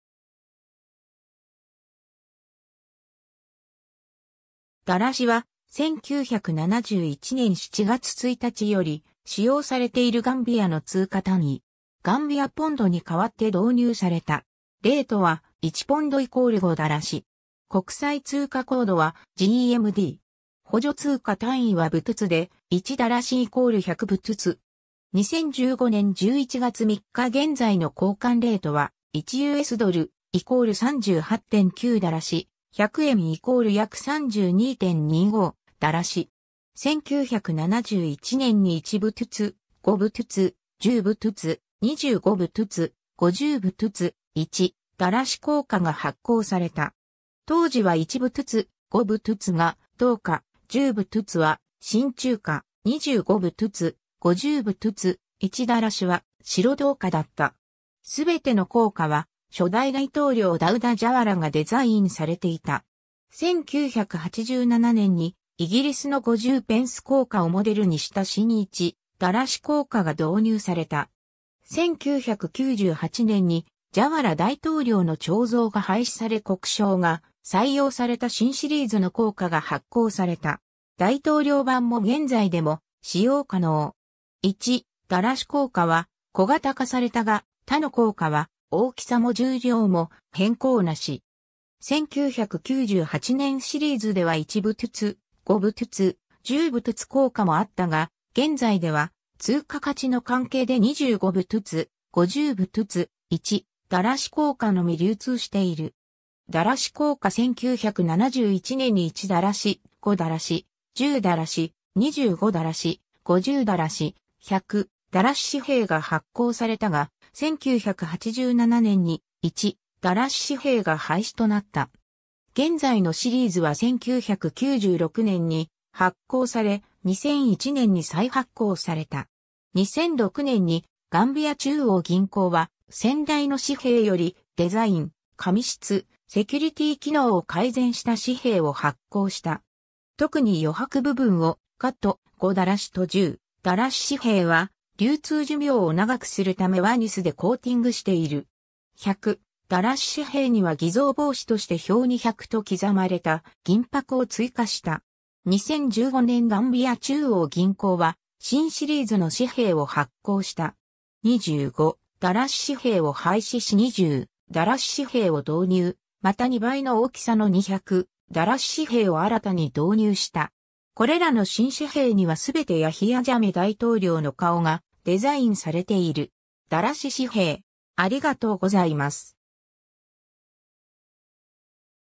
ダラシ
ダラシ（Dalasi）は、1971年7月1日より使用されているガンビアの通貨単位。
ガンビア・ポンドに代わって導入された。
レートは、1ポンド＝5ダラシ。
 国際通貨コード(ISO 4217)は、GMD。
補助通貨単位はブトゥツ（Bututs）で、1ダラシ＝100ブトゥツ。
 2015年11月3日現在の交換レートは、1USドル=38.9ダラシ、100円＝約32.25ダラシ。
 1971年に1ブトゥツ、5ブトゥツ、10ブトゥツ、25ブトゥツ、50ブトゥツ、1ダラシ硬貨が発行された。
当時は1ブトゥツ、5ブトゥツが銅貨、10ブトゥツは真鍮貨、25ブトゥツ、50ブトゥツ、1ダラシは白銅貨だった。
全ての硬貨は初代大統領ダウダ・ジャワラがデザインされていた。
1987年にイギリスの50ペンス硬貨をモデルにした新1ダラシ硬貨が導入された。
 1998年にジャワラ大統領の彫像が廃止され国章が採用された新シリーズの硬貨が発行された。
大統領版も現在でも使用可能。
1ダラシ硬貨は小型化されたが、他の硬貨は大きさも重量も変更なし。
1998年シリーズでは1ブトゥツ、5ブトゥツ、10ブトゥツ硬貨もあったが、現在では通貨価値の関係で25ブトゥツ、50ブトゥツ、1ダラシ硬貨のみ流通している。
 ダラシ硬貨 1971年に1ダラシ、5ダラシ、10ダラシ、25ダラシ、50ダラシ、100ダラシ紙幣が発行されたが、1987年に1ダラシ紙幣が廃止となった。
現在のシリーズは1996年に発行され、2001年に再発行された。
2006年にガンビア中央銀行は先代の紙幣よりデザイン、紙質、セキュリティー機能を改善した紙幣を発行した。
特に余白部分をカット、5ダラシと10ダラシ紙幣は流通寿命を長くする為ワニスでコーティングしている。
100ダラシ紙幣には偽造防止として表に100と刻まれた銀箔を追加した。
 2015年ガンビア中央銀行は新シリーズの紙幣を発行した。
25ダラシ紙幣を廃止し20ダラシ紙幣を導入、また2倍の大きさの200ダラシ紙幣を新たに導入した。
これらの新紙幣にはすべてヤヒヤ・ジャメ大統領の顔がデザインされている。
 ダラシ紙幣